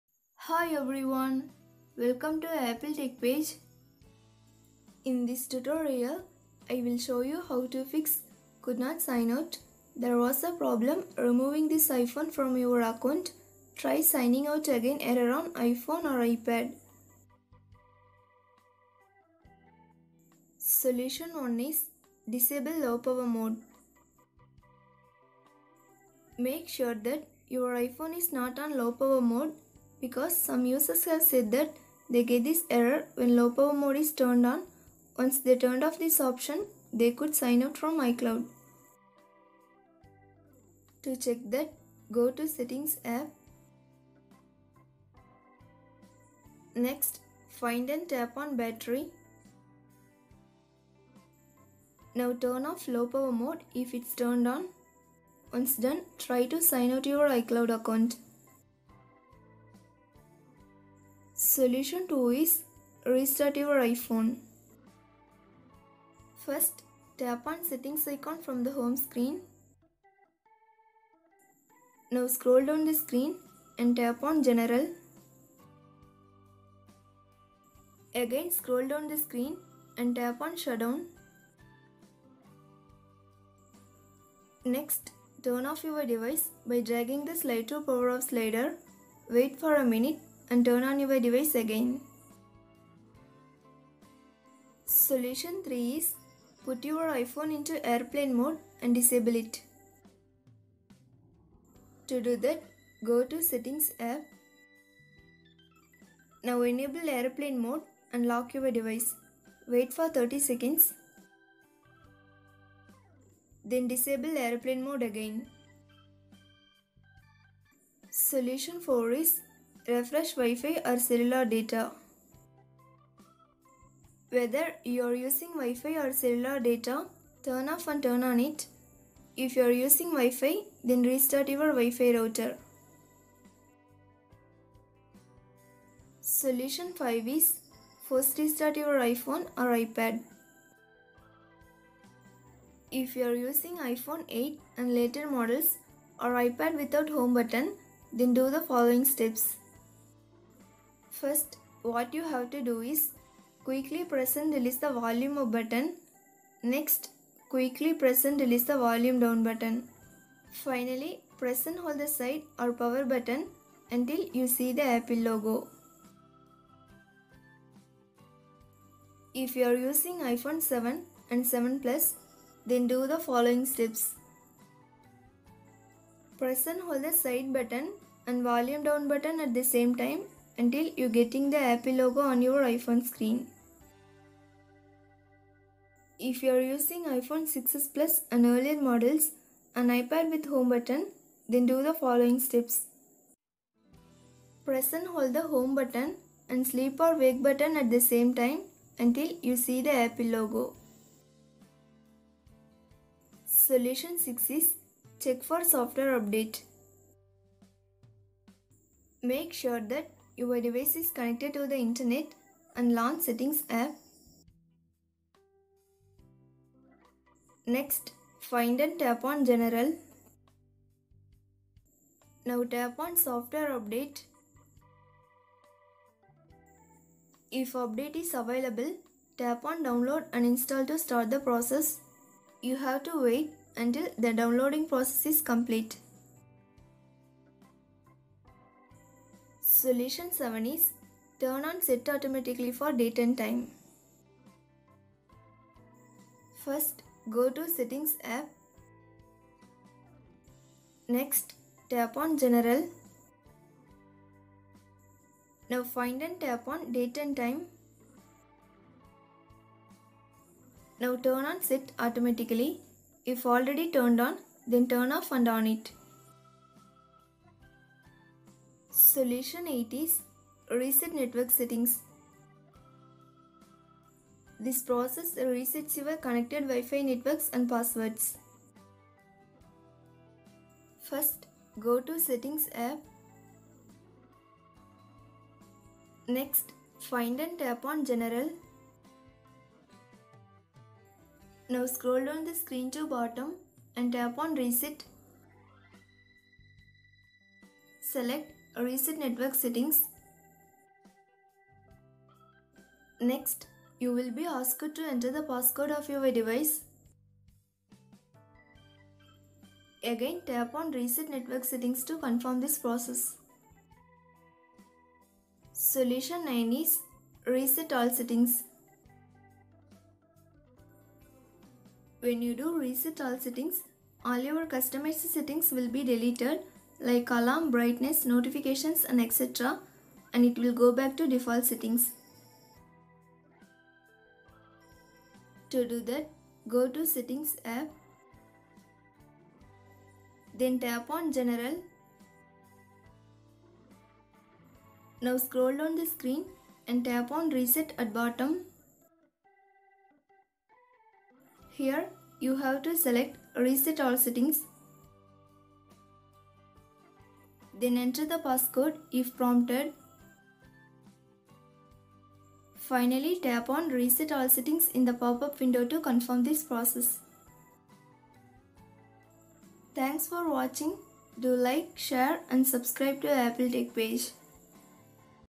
Could Not Sign Out of iCloud There was a Problem Removing this iPhone from your Account in iOS 13.4
How to fix Could not sign out of iCloud in iOS 13, can't sign out of my Apple Id on iPhone, cannot sign out of Apple ID on iPhone in iOS 13.3, cannot sign out of iCloud because phone is being restored.